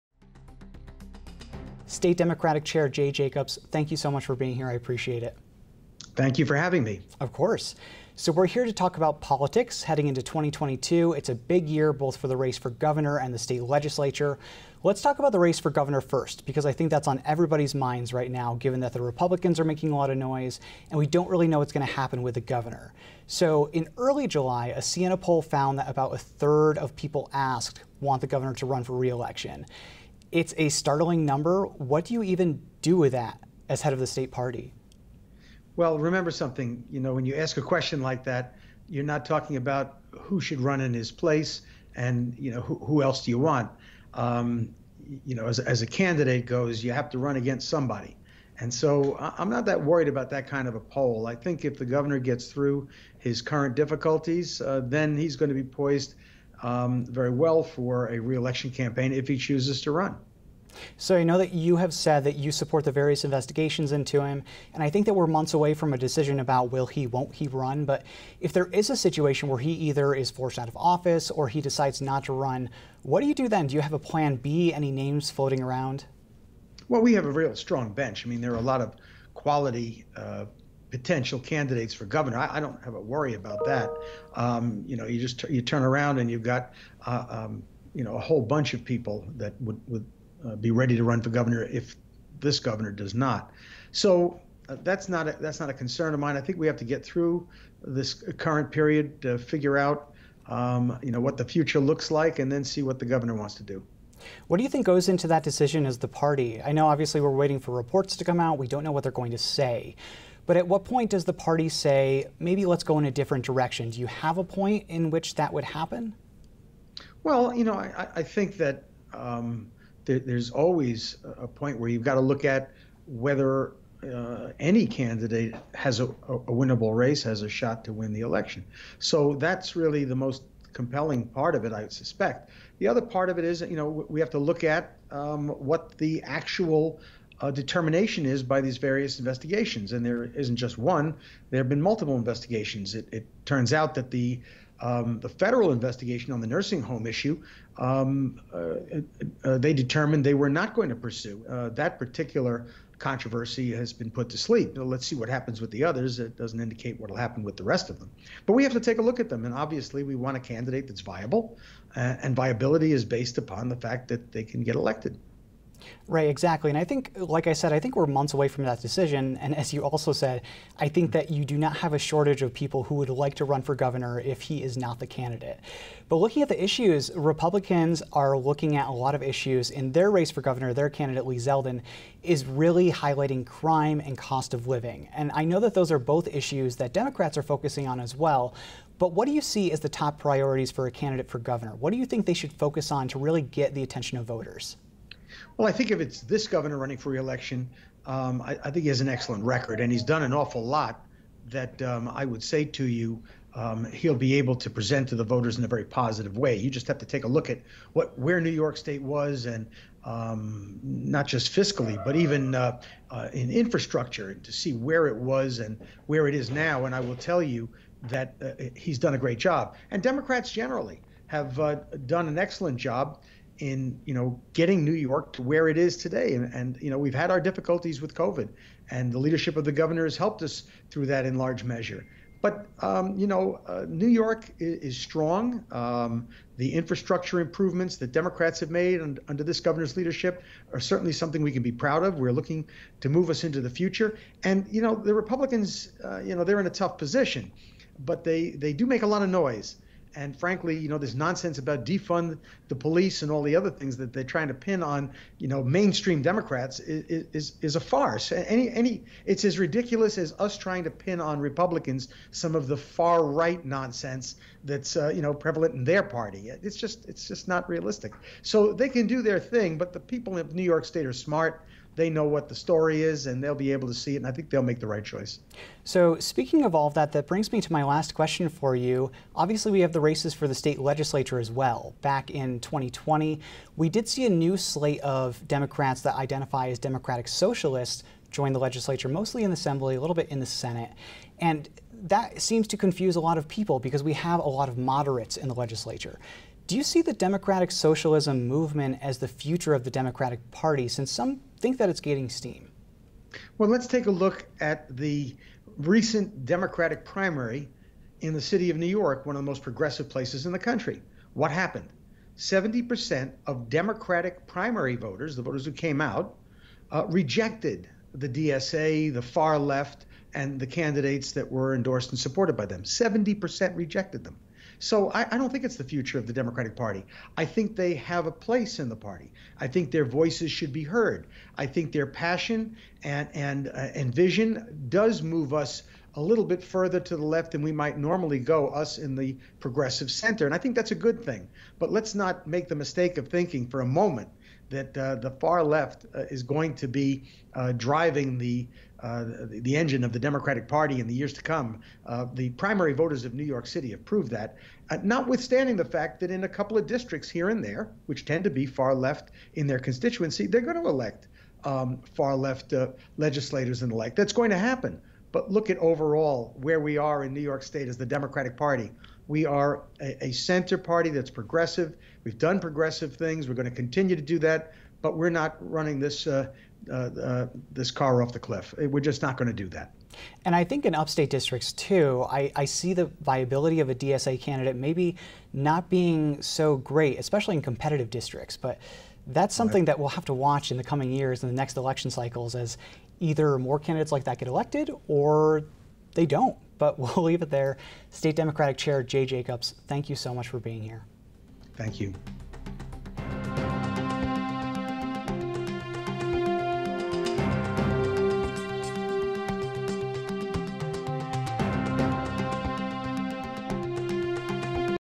State Democratic Chair Jay Jacobs on Party Strategy, Elections | New York NOW
State Democratic Chair Jay Jacobs joins to discuss the party's strategy in next year's elections, including the race for governor.